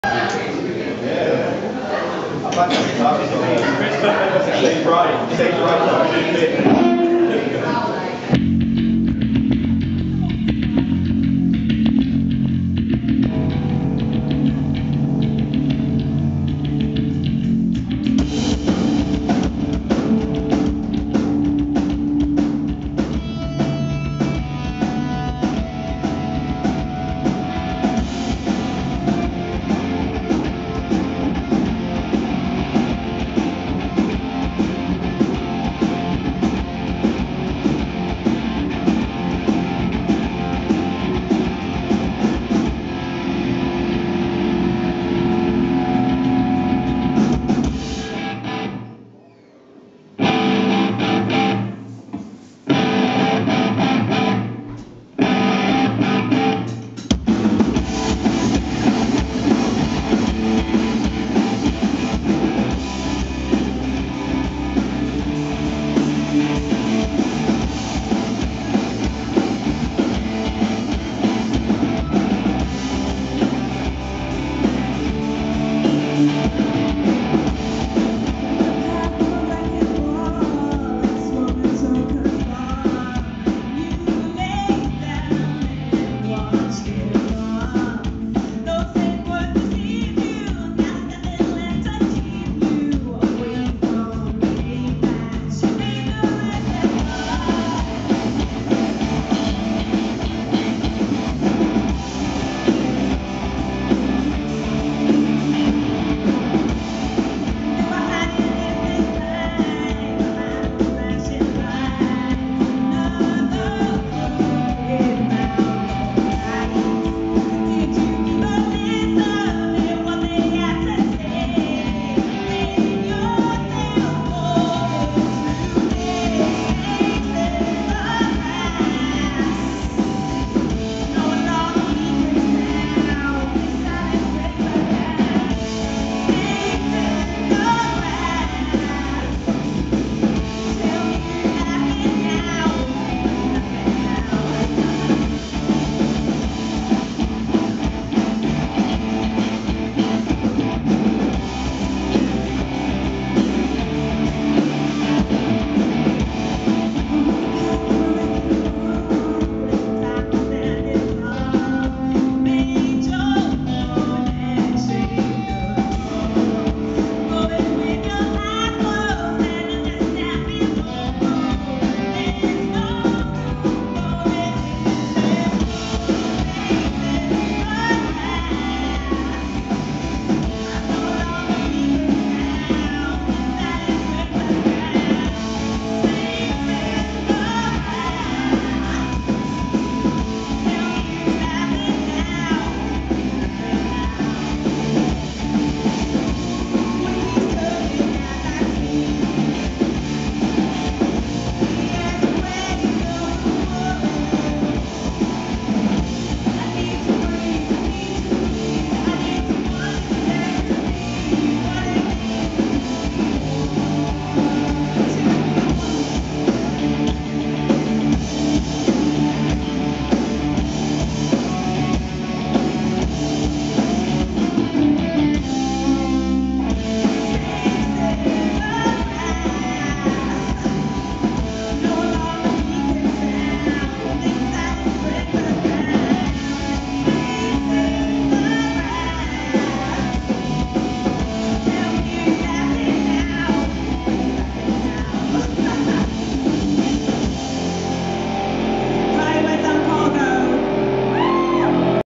Damn Cargo - The Hairy Dog, Derby - 3rd February 2018
Damn Cargo performing at The Hairy Dog, Derby on 3rd February 2018